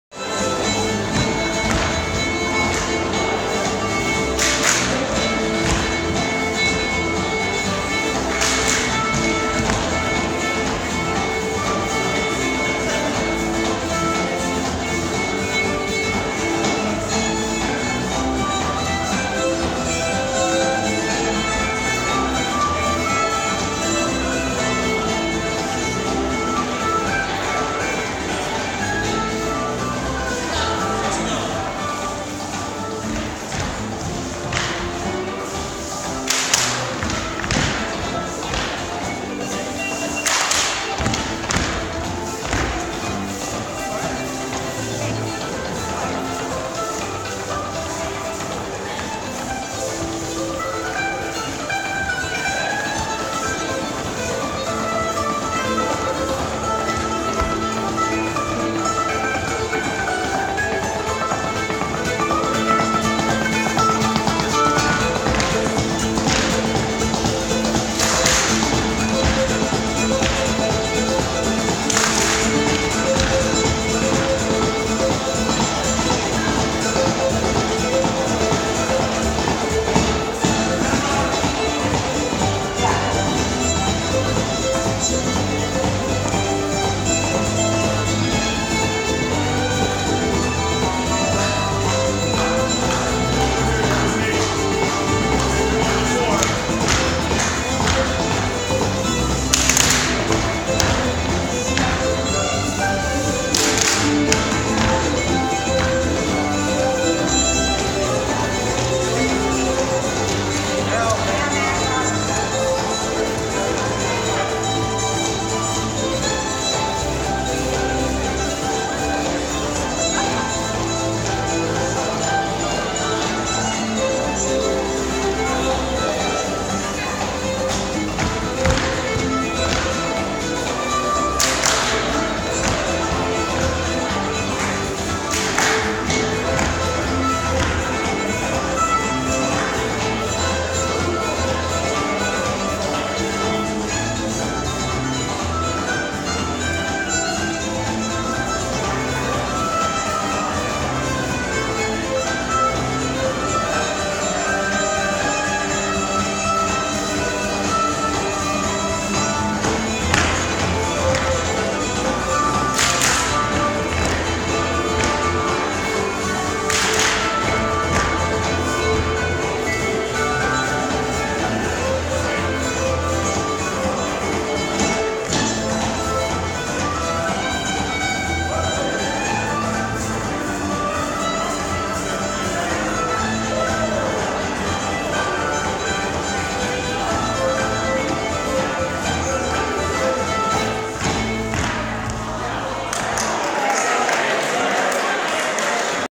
Pumpkin Spice Madness Contra Dance - A1 Petronella var.
Laura Johannes calls Don Veino's dance "Pumpkin Spice Madness" at the Monday Contras series at the Concord (MA) Scout House. This is the 2x Petronella variation (A1).

Music by Eric Boodman (fiddle, foot percussion), Donal Sheets (cello, guitar, foot percussion) & Jack O'Connor (mandolin, banjo, percussion).